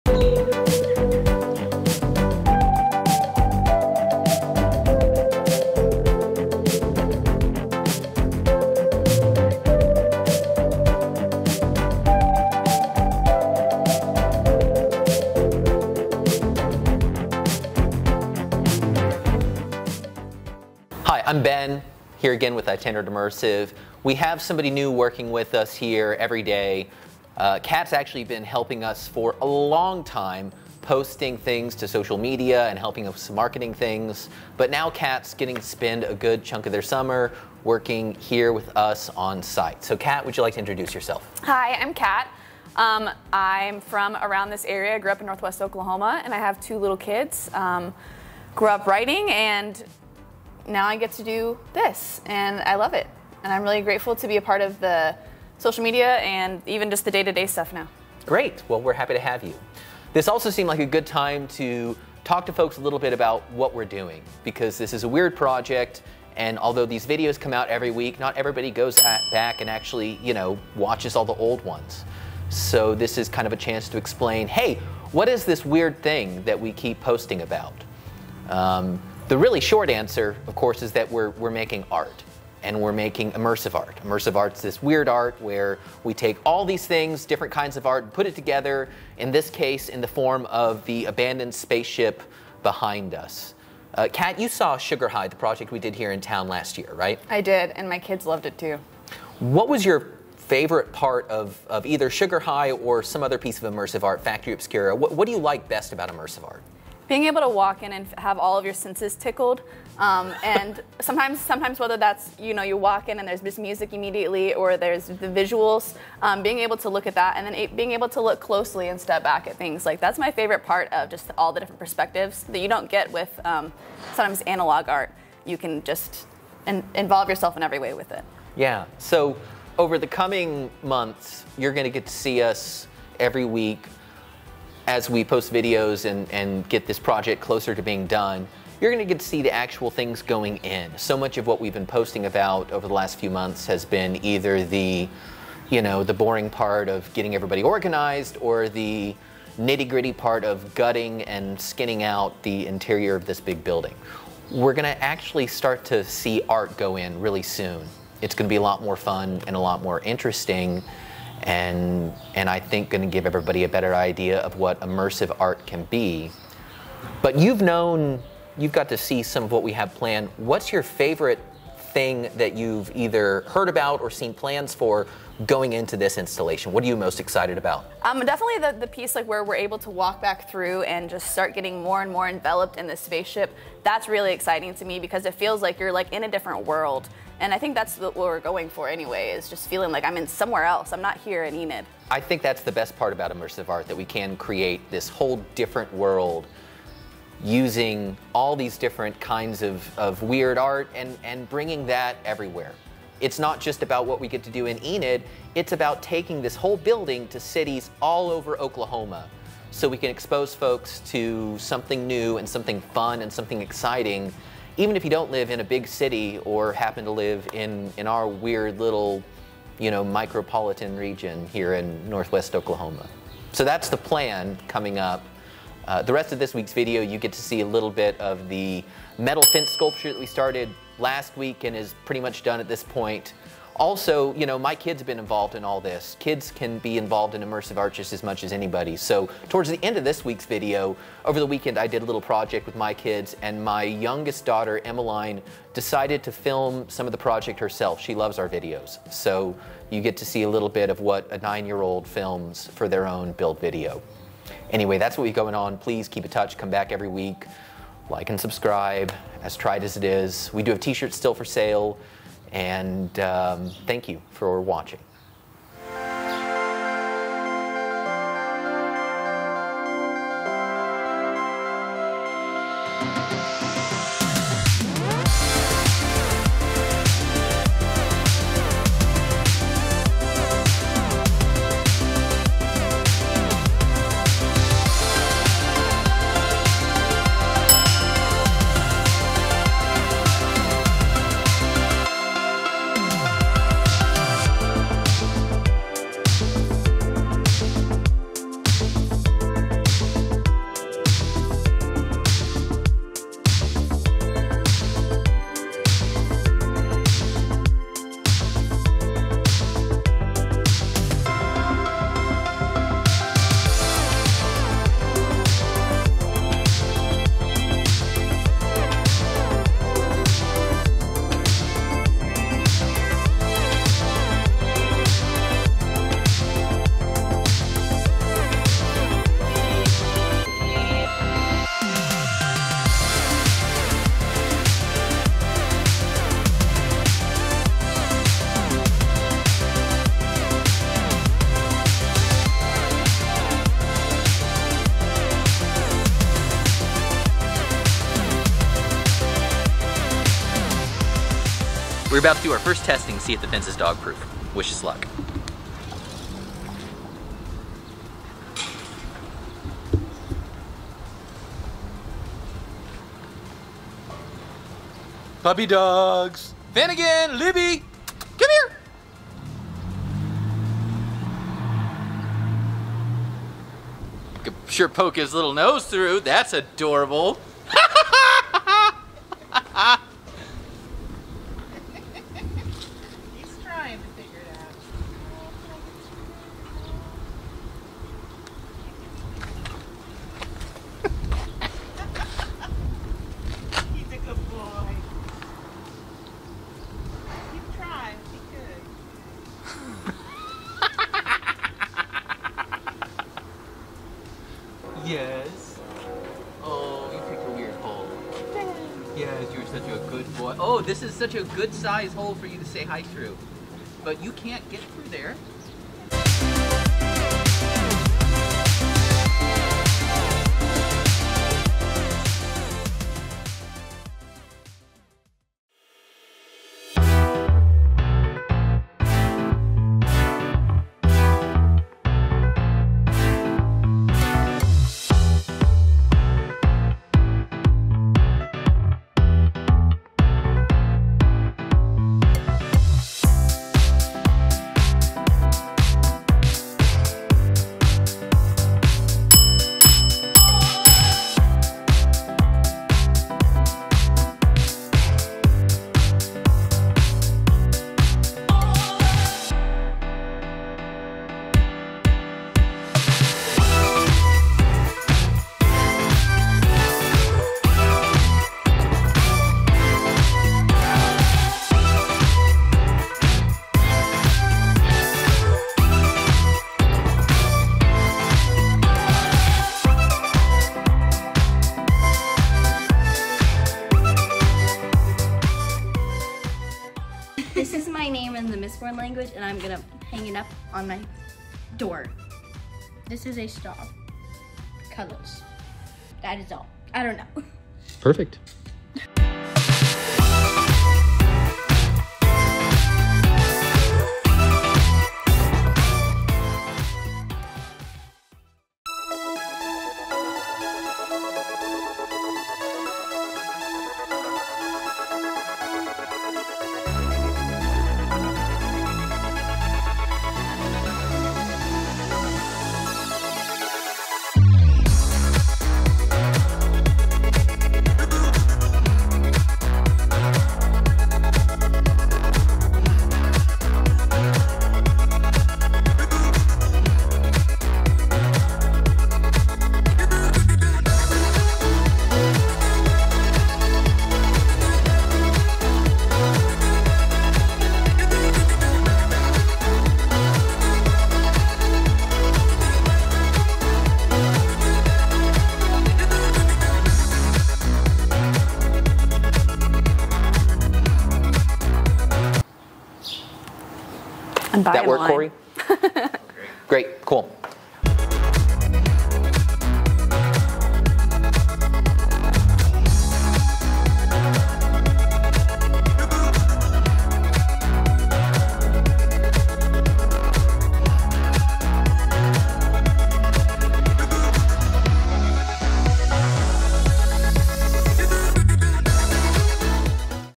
What Are We Doing?!
Let’s all welcome Kat to Itinerant Immersive!

Well actually Kat has been helping us out for quite a while behind the scenes doing social media posts and marketing stuff, but now they are here on site and getting their hands dirty!

Also in this week’s video take a look at the finished metal sculpture that we showed the first part of last week. It’s done now, dog proof, and creates the coolest shadows ever.

Ben’s kids spent a day learning to use the ShopBot and Emeline decided to film her own build video, stick around to the end to check that out.

The front of the ship is coming along, with Rowan creating a 3D model of the intended design so we can get the geometry just right for the ShopBot.